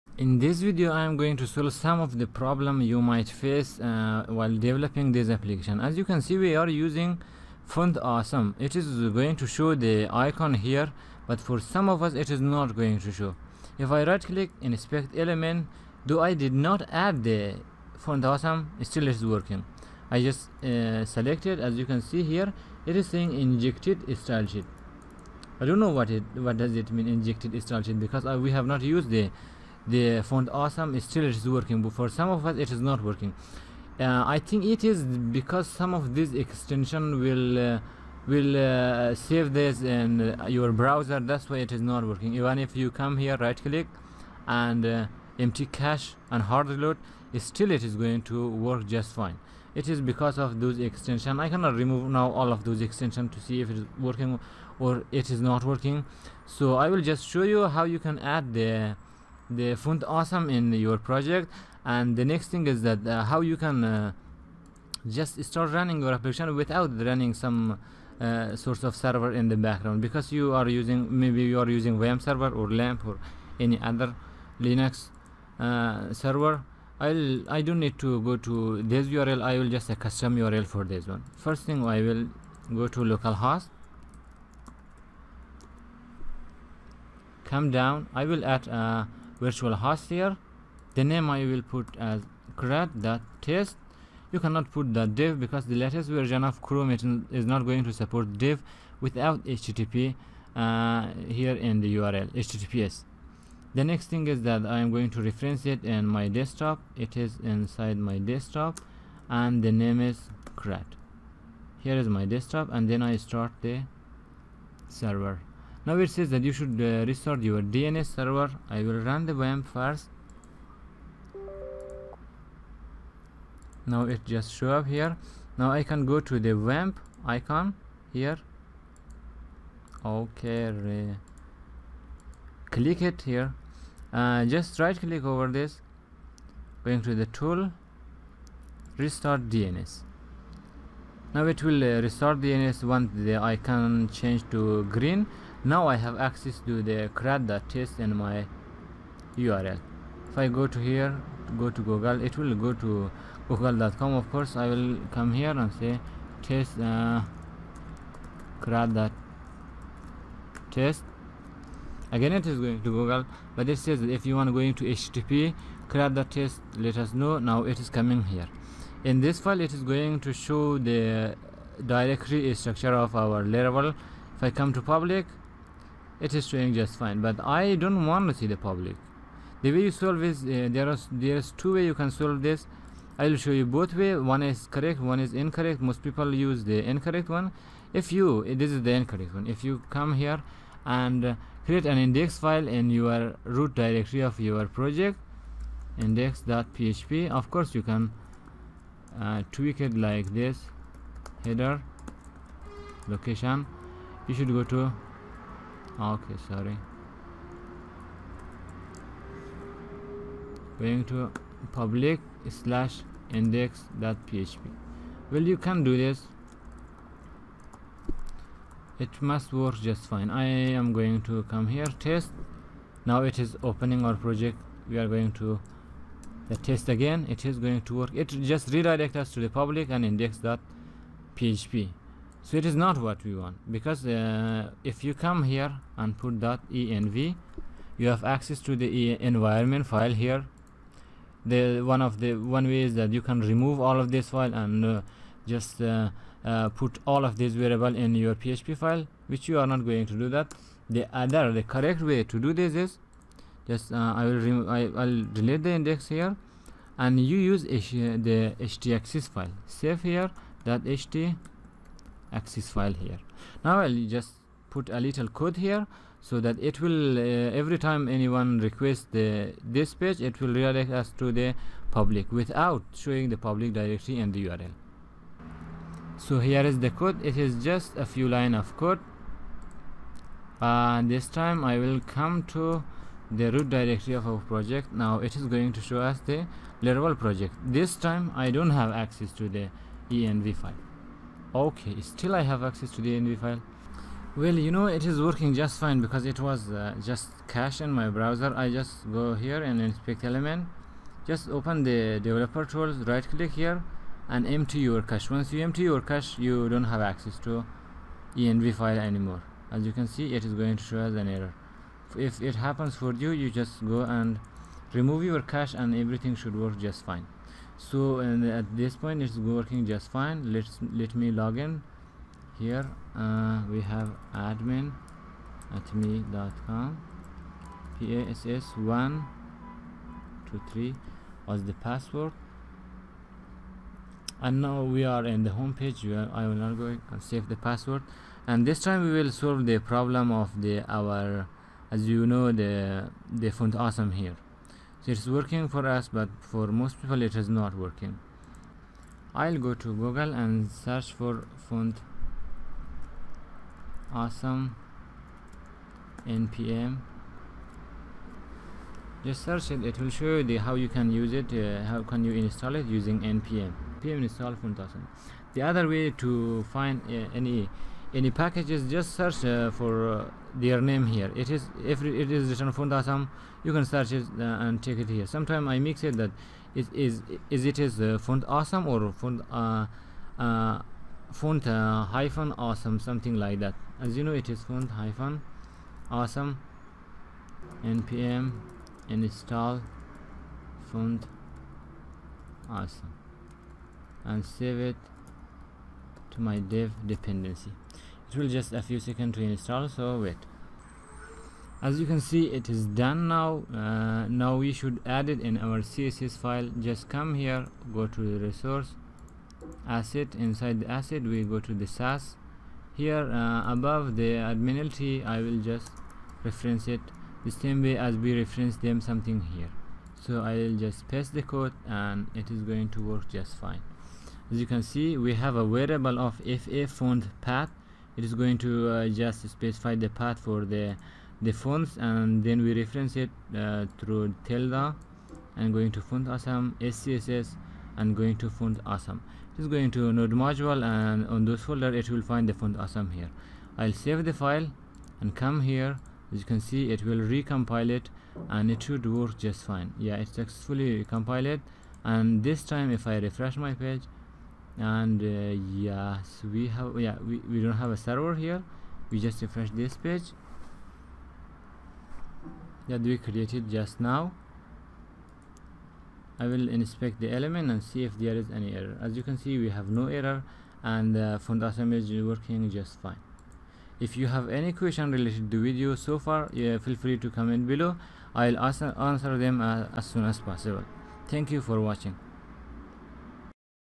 03 How to Solve Font Awesome Issue with Admin LTE in Laravel 5 5
If you have any issue with font awesome and Admin LTE and laravel, this video will help you solve those problems. Sometimes, AdminLTE and font-awesome have some issues, we will solve it in this video, Also, we will install it with npm and add it to our css files.